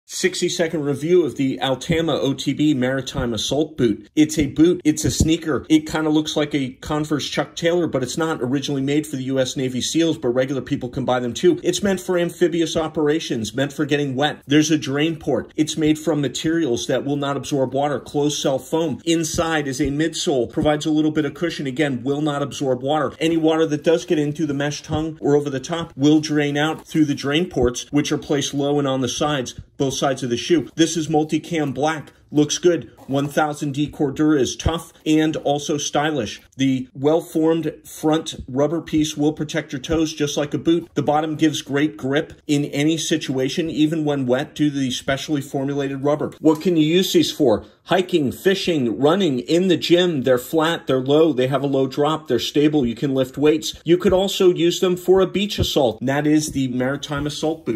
Altama OTB Maritime Assault Boot Mini Review
This is a short mini-review on the Altama OTB Maritime Assault Boot - use it as a cross-trainer, or a boot - it's a good looking shoe.